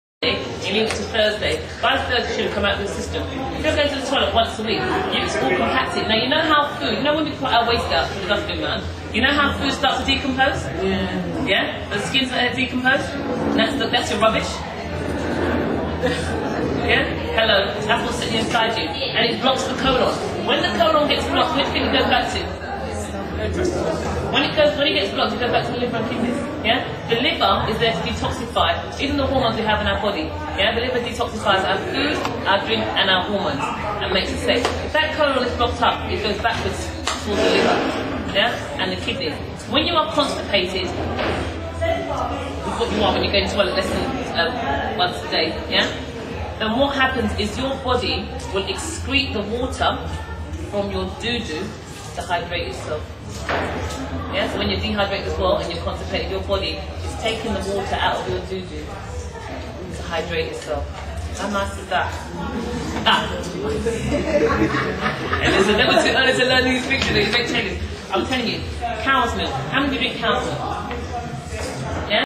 3ness - Seminar - Jenni Russell
Jenni Russell explains about the digestion system and tells it how it is.